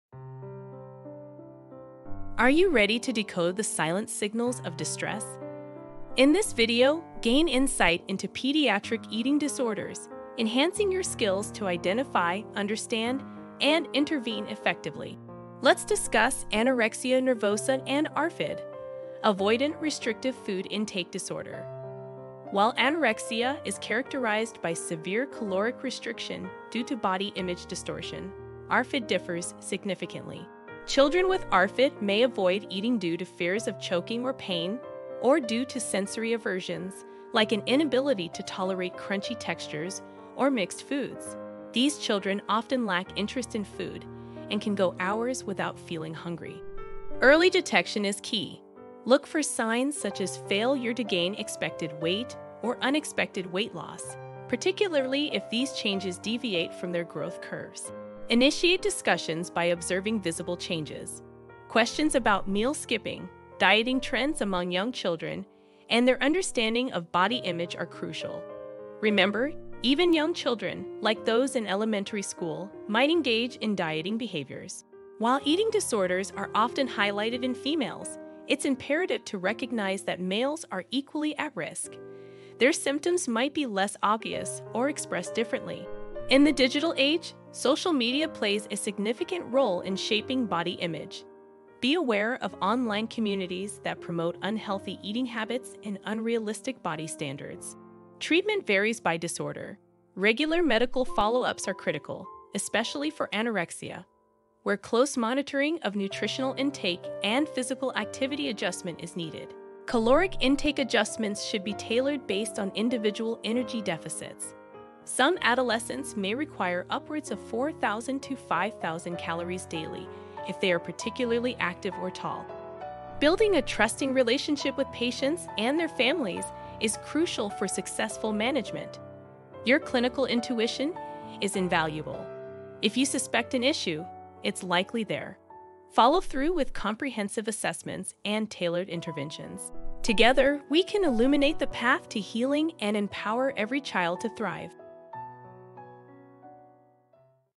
Understanding Pediatric Eating Disorders | MedAnimations
Explore the complexities of pediatric eating disorders. Discover key insights on identifying and treating conditions like Anorexia Nervosa and ARFID, and learn how early detection and tailored interventions can transform outcomes. Join us to enhance your skills and empower every child to thrive in their journey to recovery.